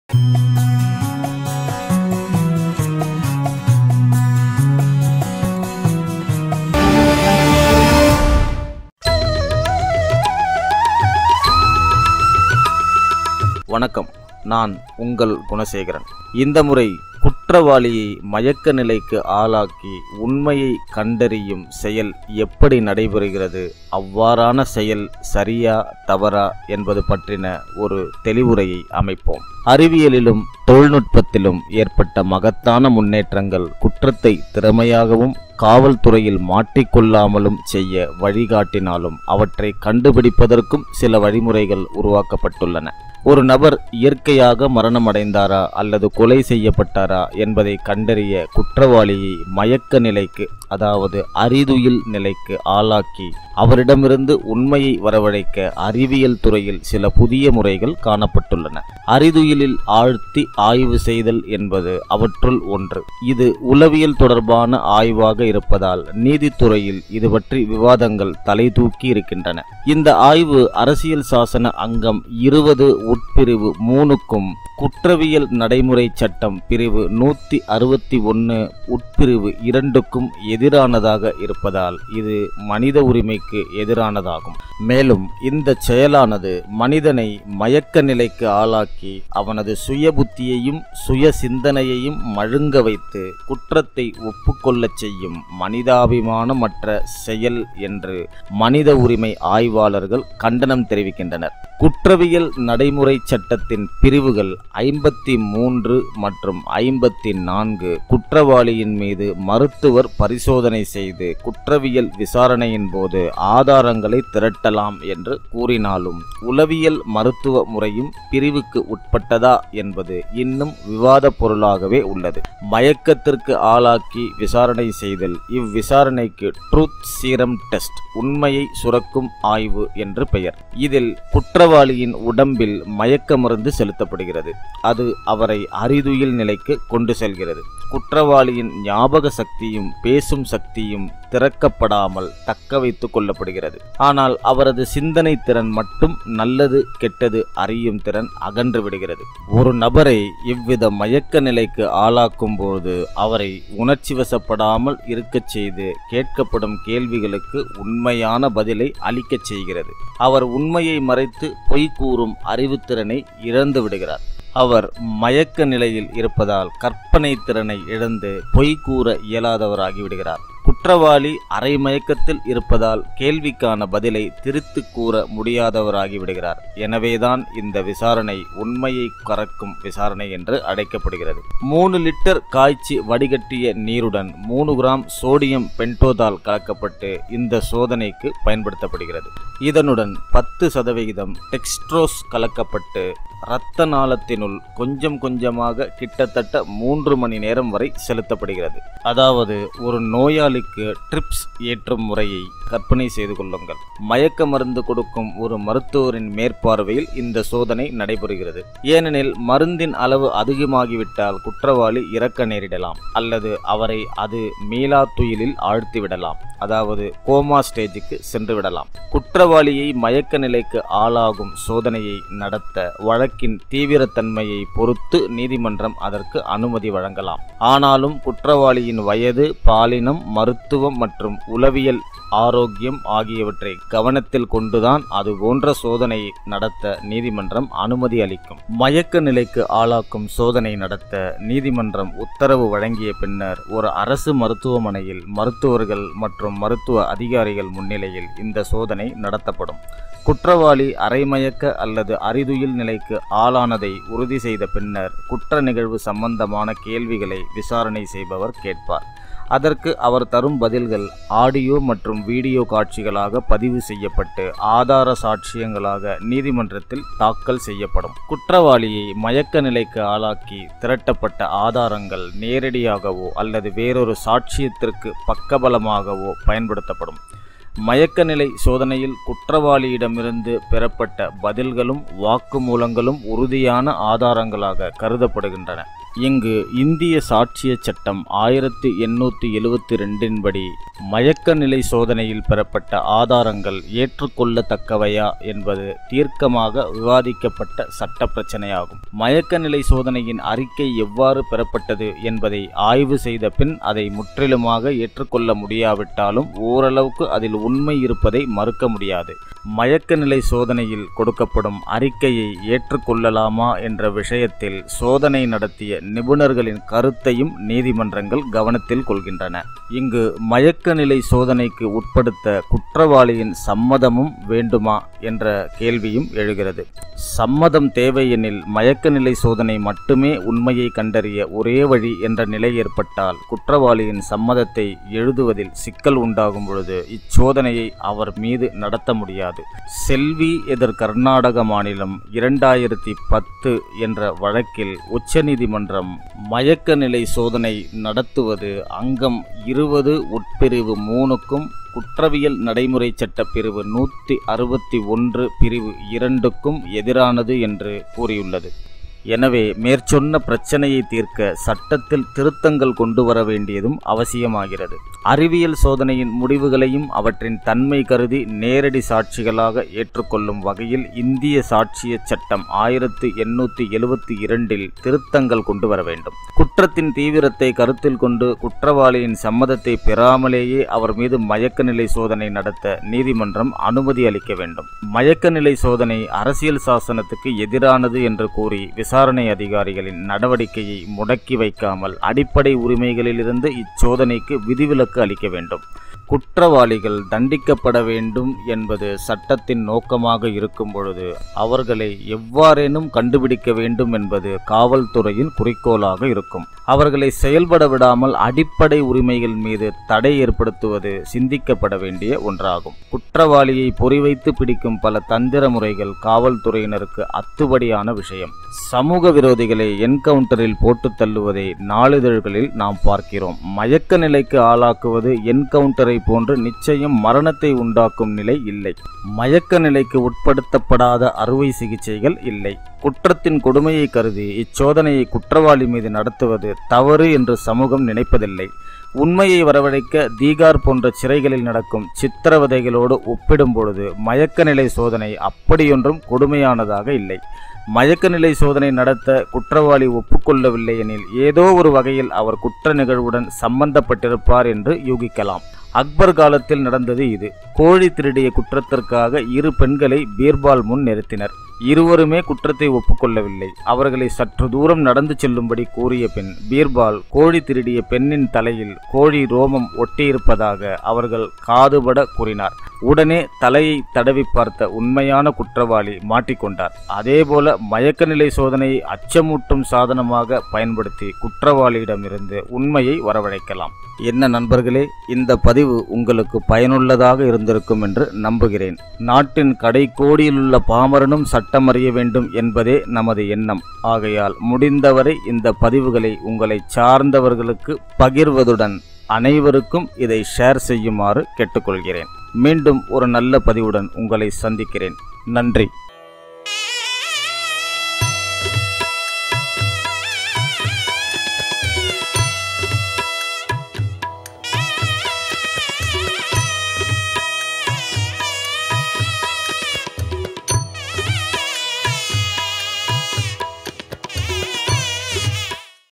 குற்றவாளியை மயக்க நிலைக்கு ஆளாக்கி உண்மையை கண்டறியும் சோதனை? Is the truth-finding test right? Wrong?
Truth_Serum_Test Truth_finding_test LawyersLine குற்றவாளியை மயக்க நிலைக்கு ஆளாக்கி உண்மையை கண்டறியும் சோதனை சரியா? தவறா? : Is the truth-finding test right? Wrong?

Truth Serum Test (உண்மையை கரக்கும் ஆய்வு)
Sodium Bendothol and Textrose

Advocate : Madras High Court

State President : FFLAAC (Federation for free legal aid and anti corruption)